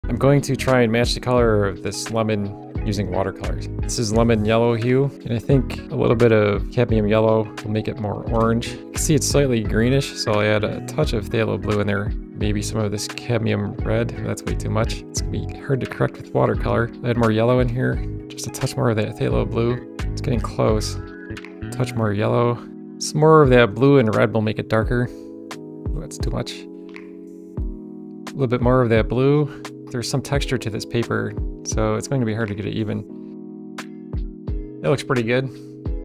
Lemon Yellow Isn't Actually the Color of Lemons? 😂
I added way to much red at one point. It’s difficult to make corrections with watercolor, especially if you’re using staining pigments. Of the three watercolor demos I posted, this one was the hardest.